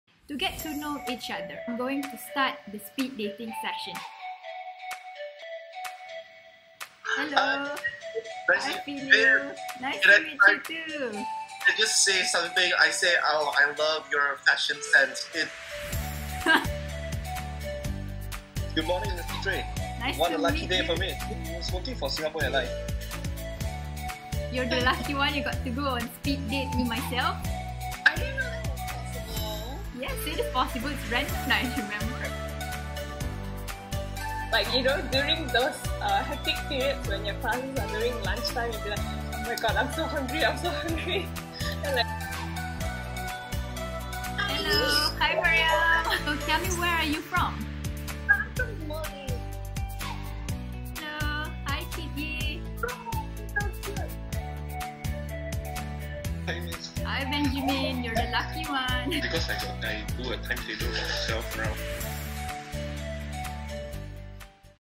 When a law lecturer goes on a speed date with her law students
This is part of virtual class activity to assess students' oral presentation skills and also their ability to advocate or pitch their points within a duration of 5 minutes.